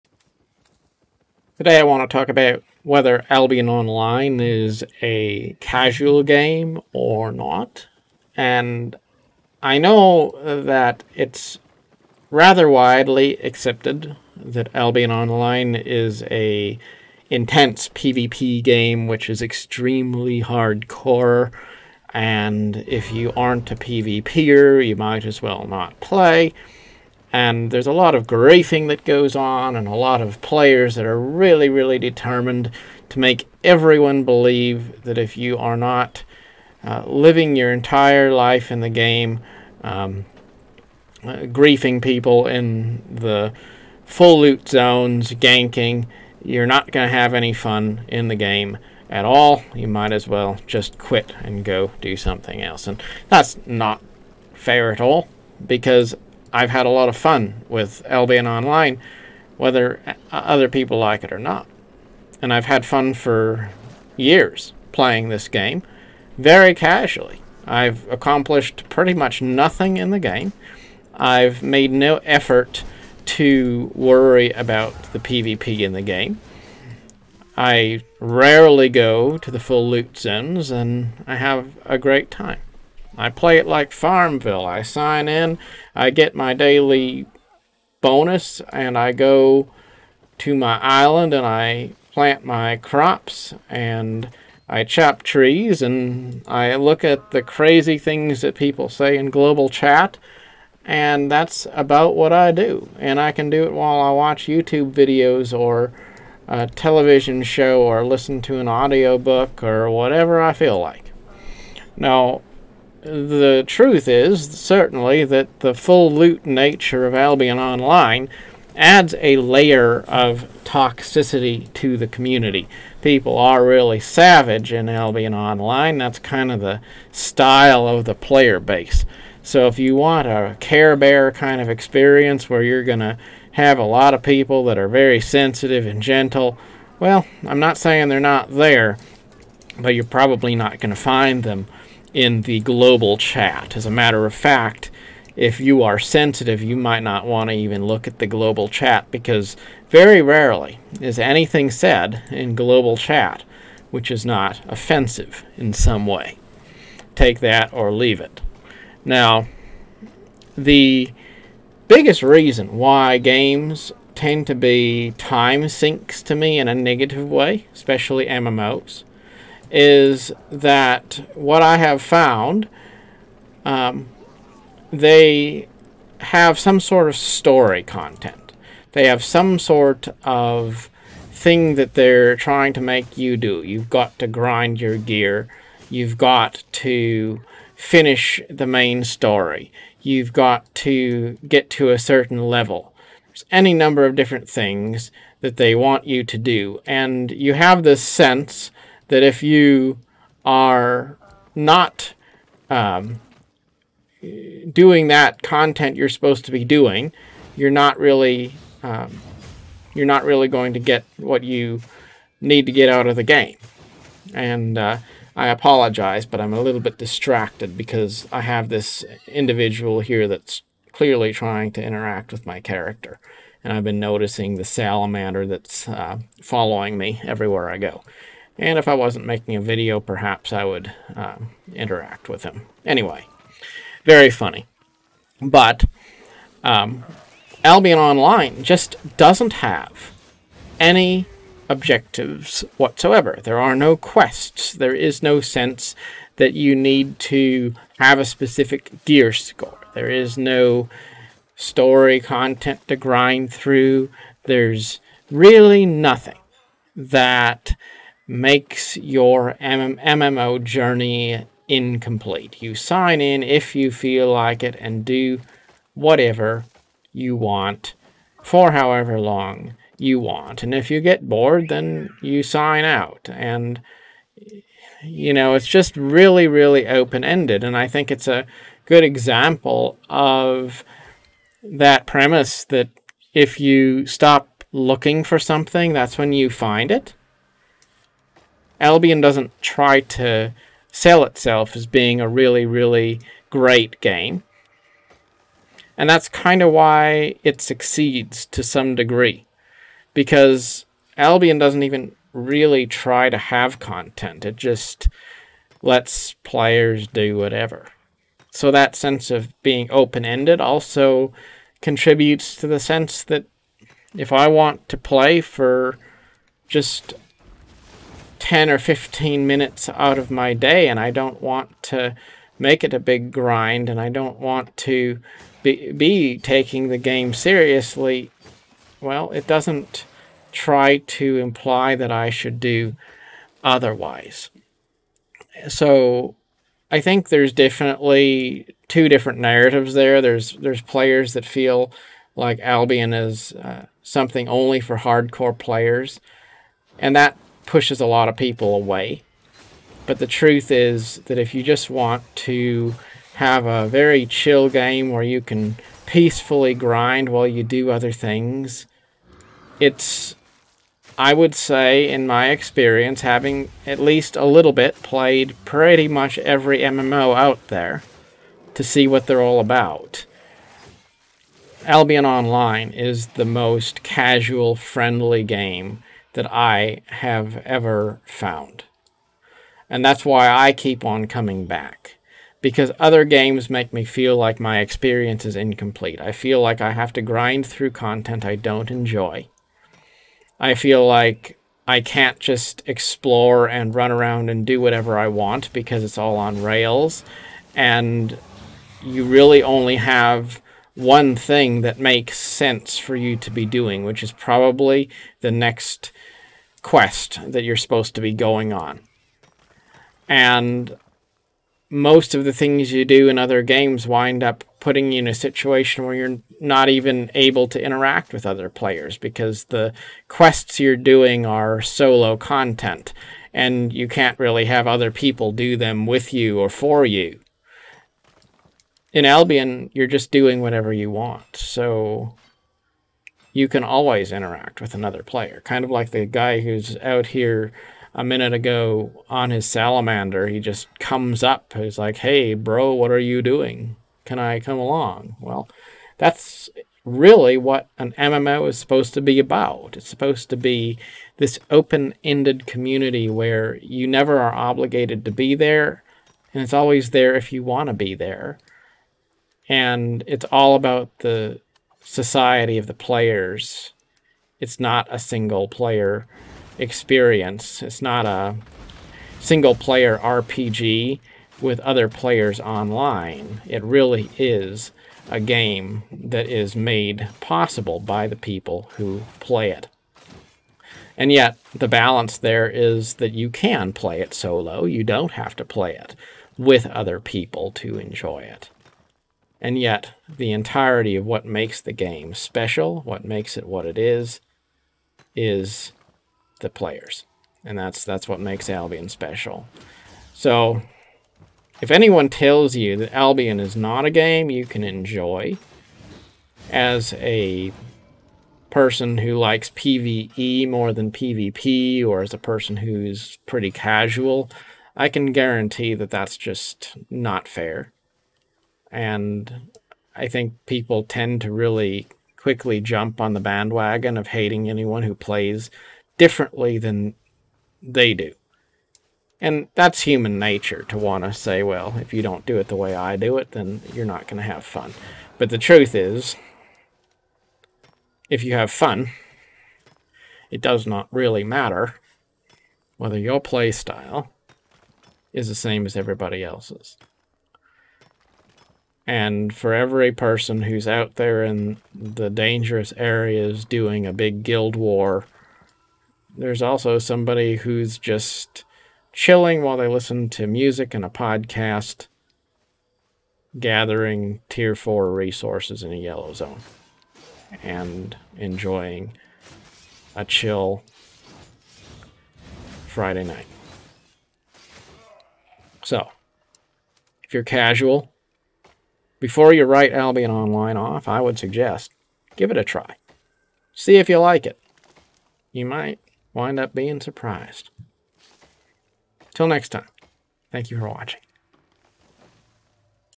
Is Albion Online Casual-Friendly?
Lots of people will tell you that Albion isn't for Casuals because it has Full-Loot PVP.  But the truth is, you can spend many hours chilling in Albion farming, gathering, and doing whatever you please in an open-ended way without ever worrying about the more hardcore content.  Albion is a game you can play your way.  Before you write it off, give it a try!